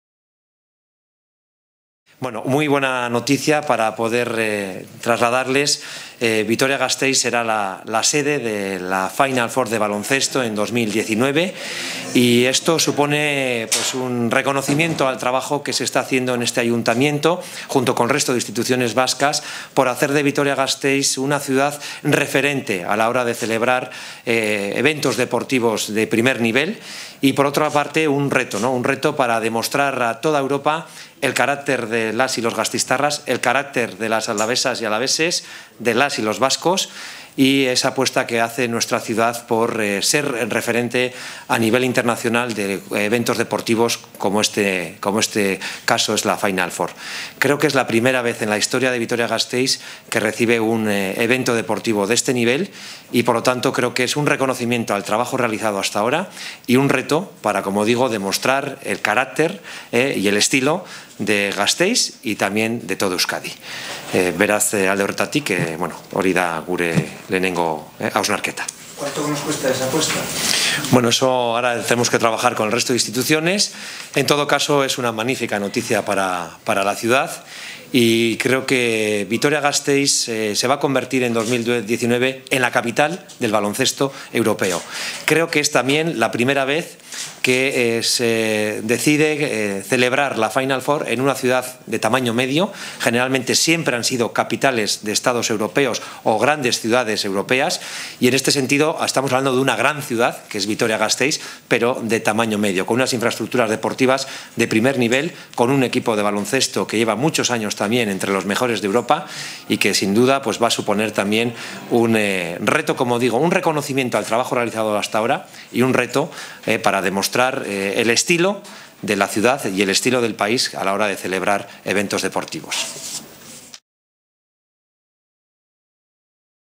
Vitoria Gasteiz acogerá la Final Four de la Euroliga en 2019
La Final Four de la Euroliga del próximo año 2019 se celebrará en Vitoria-Gasteiz. Es una noticia estupenda y que por fin se hace realidad. Supone un reconocimiento al trabajo realizado por el Ayuntamiento y el resto de las instituciones vascas. Y también va a ser un reto para demostrar a Europa que somos un referente a nivel deportivo.